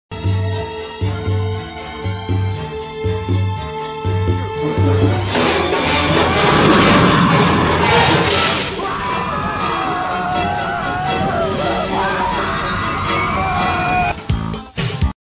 MEME POLÍTICO (O susto do leite condensado do Bolsonaro 1!) - Exxocet 1428
Contexto: Eleições no Congresso afetam o futuro do governo Bolsonaro.
Período: 27/01/2021
Local: Brasília / DF